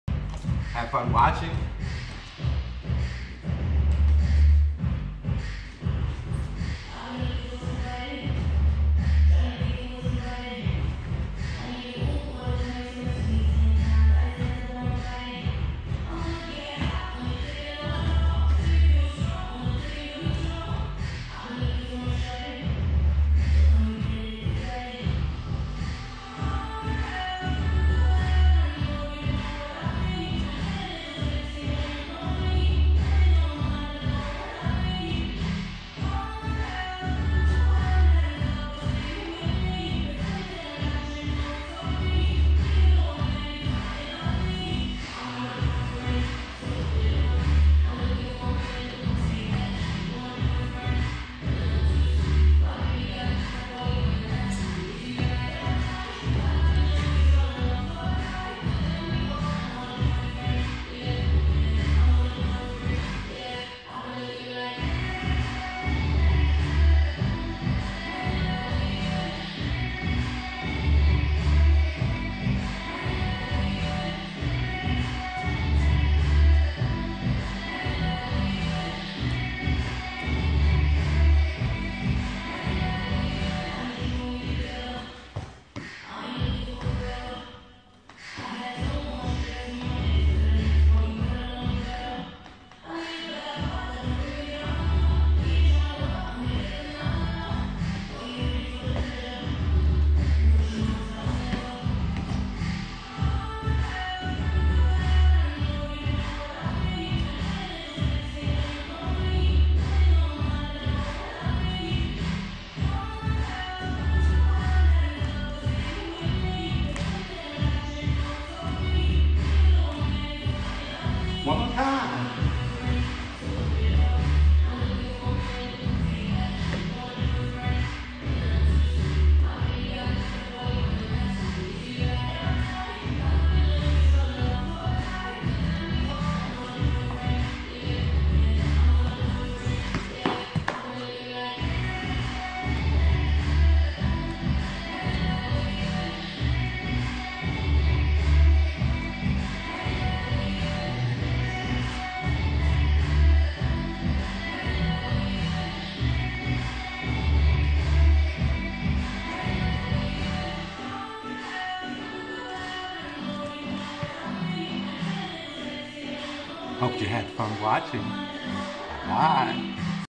DanceHall DERRICK
Online Dancing with Derrick Odell Jackson @derrick_o._jackson
warmup and choreography for dancersover40 👍 .